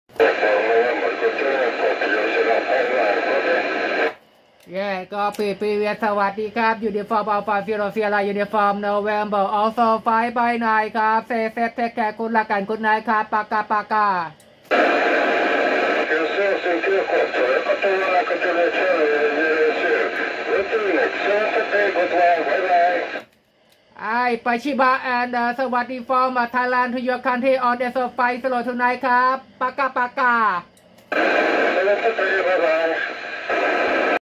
UA0SUN fm qso with E21EJC at my AOS time on Satellite SO-50, 25 FEBRUARY 2022
at my AOS time 13:19 utc,Thanks qso cu again